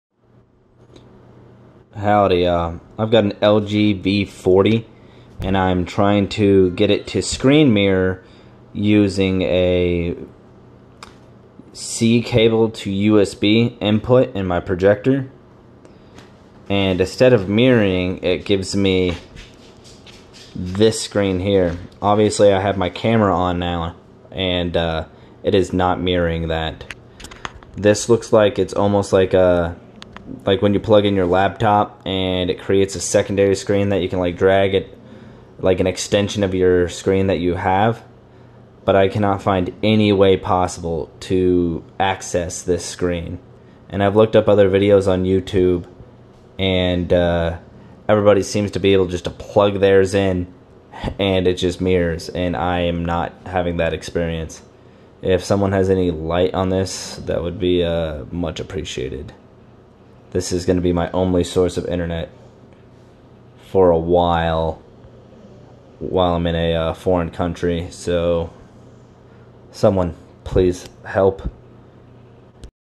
LG V40 ThinQ C cable to HDMI screen mirroring not working
getting secondary screen I can't get to